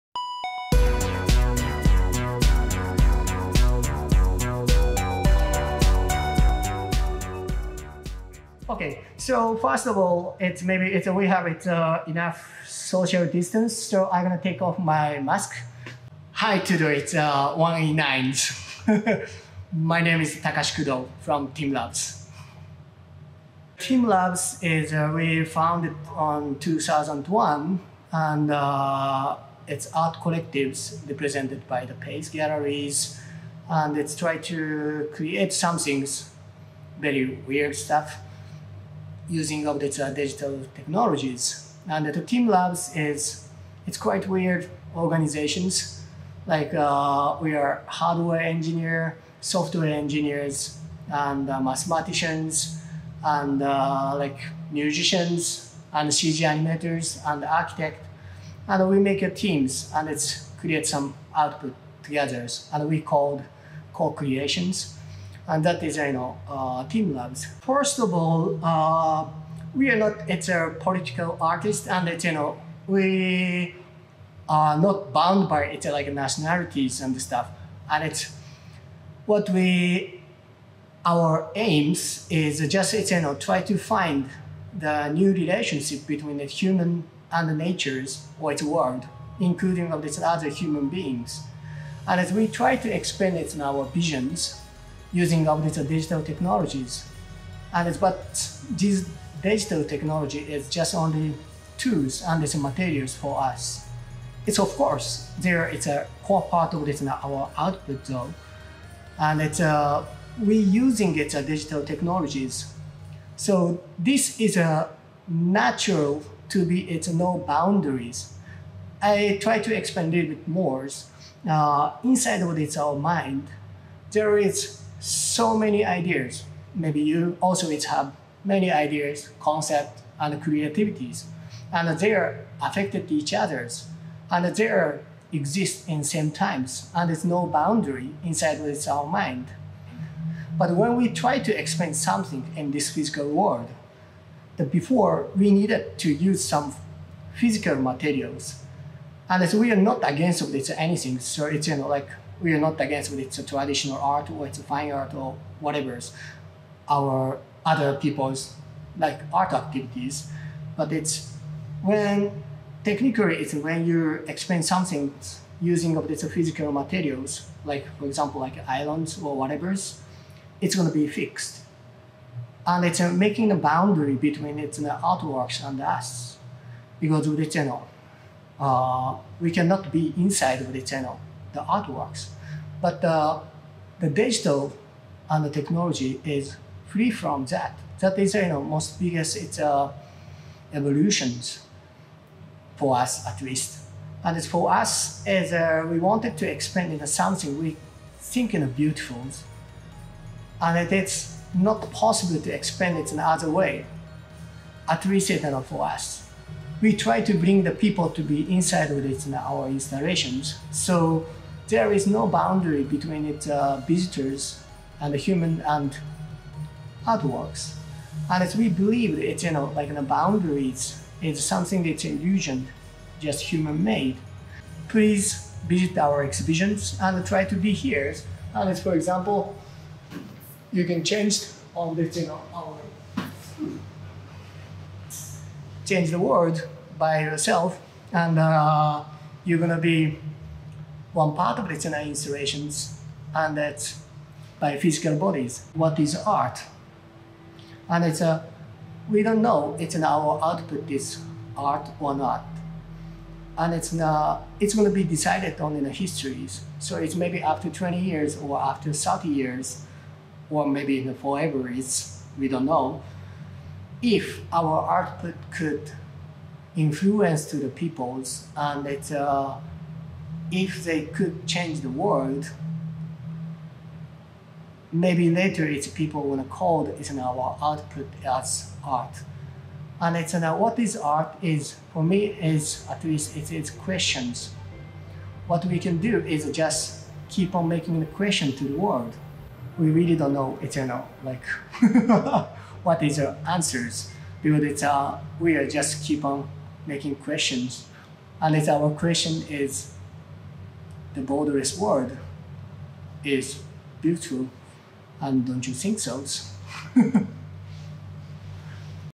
Takashi Kudo (teamLab) - Borderless: Artworks Without Boundaries
Keynote by Takashi Kudo from teamLab at 1E9 THE_CONFERENCE 2020.